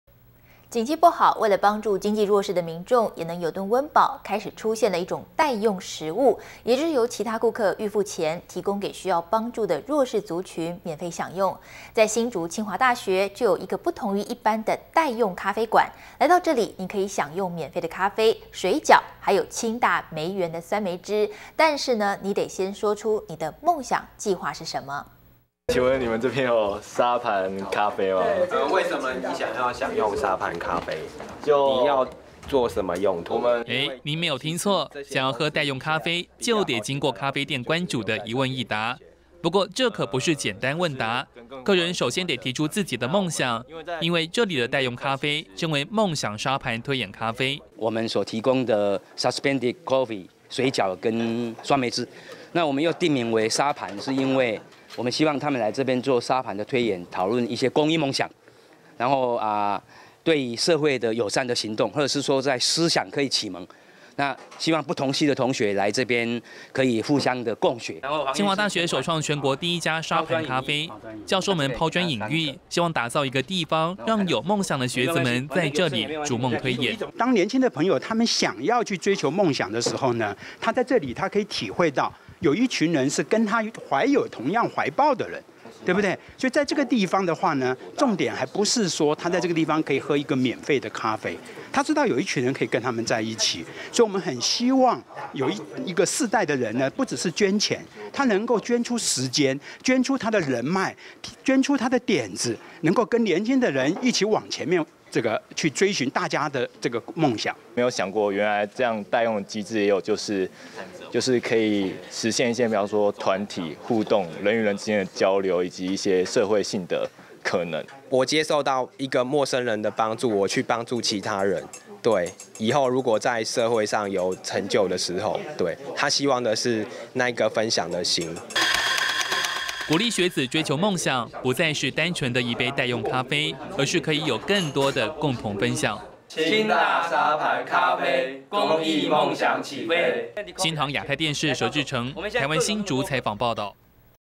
【公益夢想_台灣新聞】喝咖啡推演夢想 清大另類待用咖啡
【公益夢想_台灣新聞】喝咖啡推演夢想 清大另類待用咖啡(NTDAPTV) 景氣不好，為了幫助經濟弱勢的民眾也能有頓溫飽，開始出現一種「待用」食物，也就是由其他顧客預付錢，提供給需要幫助的弱勢族群免費享用。在新竹清華大學，就有一個不同於一般的「待用」咖啡館，來到這裡你可以享用免費的咖啡、水餃還有清大梅園的酸梅汁，但是你得先說出你的夢想計畫是什麼。【新唐人亞太台2013年4月23日訊】

新竹清華大學 曹同學：「請問你們這邊有沙盤咖啡嗎？」

咖啡店負責人 郭建宏：「為什麼你想享用沙盤咖啡？你有什麼用途？」

你沒有聽錯，想要喝待用咖啡，就得經過咖啡店關主的一問一答，不過這可不是簡單問答，客人首先得提出自己的夢想，因為這裡的待用咖啡稱為夢想沙盤推演咖啡。

新竹清華大學 王俊秀教授：「我們所提供的這個Suspended Cafe(待用咖啡)，水餃跟酸梅汁，那我們又定名為沙盤，是因為我們希望他們來這邊做沙盤的推演，討論一些公益夢想，然後對於社會有善的行動，或是說在思想可以啟蒙，希望不同系的同學來這邊可以互相的共學。」

清華大學首創全國第一家沙盤咖啡，教授們拋磚引玉，希望打造一個地方，讓有夢想的學子們，在這裡逐夢推演。

中央研究院院士 黃一農：「當年輕的朋友他們想要追求夢想的時候，他在這裡他可以體會到，有一群人是跟他懷有同樣懷抱的人，在這個地方重點還不是說，他在這地方可以喝一個免費的咖啡，他知道有一群人可以跟他們在一起，所以我們很希望，有一個世代的人，不只是捐錢，他能夠捐出時間、捐出他的人脈，捐出他的點子，能夠跟年輕的人 一起往前面去追尋 大家的夢想。」

新竹清華大學 曹同學：「沒有想過原來待用機制，也有就是，就是實現一些團體、互動人與人的交流以及社會性的可能。」

咖啡店負責人 郭建宏：「我接受到一個陌生人的幫助，我去幫助其他人，以後如果在社會上有成就的時候，他希望的是那一個分享的心。」

鼓勵學子追求夢想，不再是單純的一杯待用咖啡，而是可以有更多的共同分享。
新唐人亞太電視 佘志誠 台灣新竹採訪報導【好樣!Formosa_帶您看見台灣的美好】